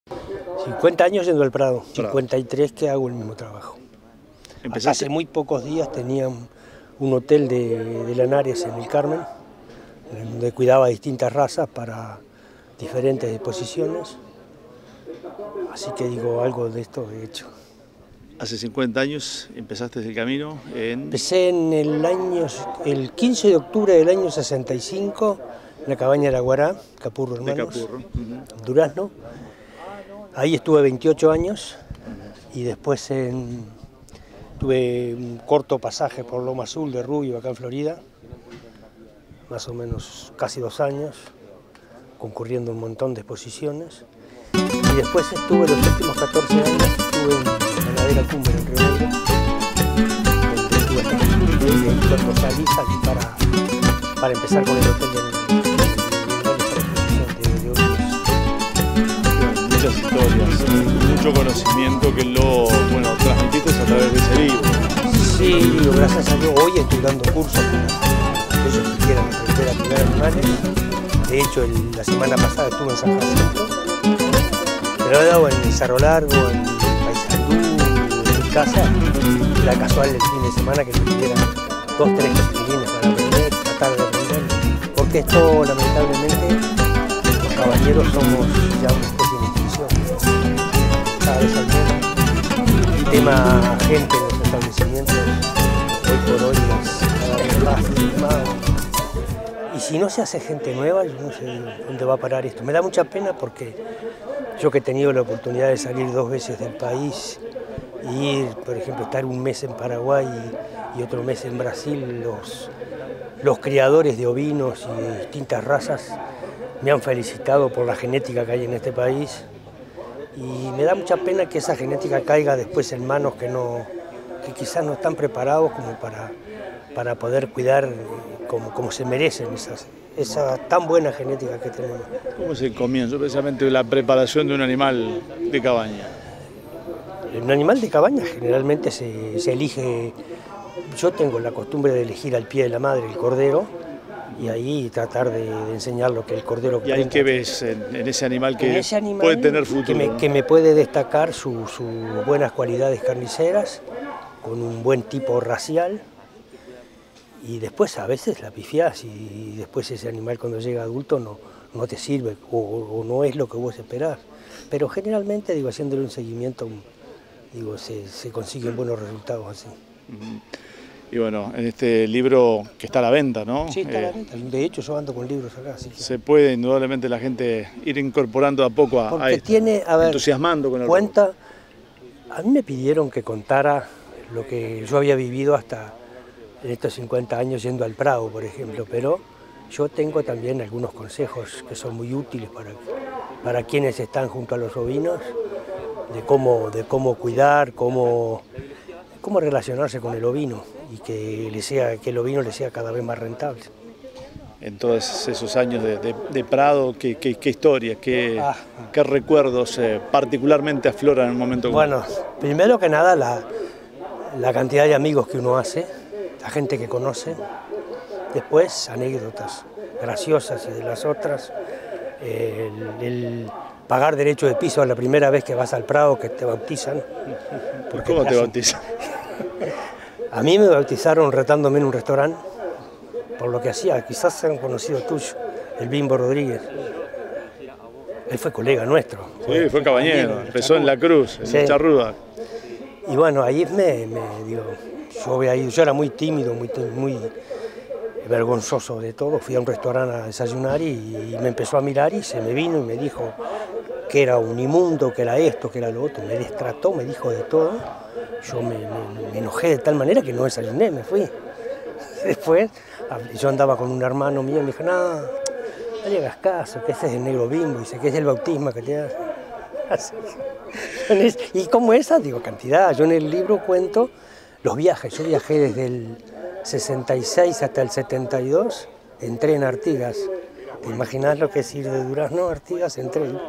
CABAÑERO "RULO GONZALEZ" - 50 AÑOS EN EL PRADO
Tras 53 años de cabañero y 50 de concurrir al Prado, Raul Heber Gonzalez Cammarano, mas conocido por “RULO”, cuenta sus historias detras de las ovejas. Una hermosa vivencia de un hombre apasionado por su profesion que resolvio contarlo a traves de un libro.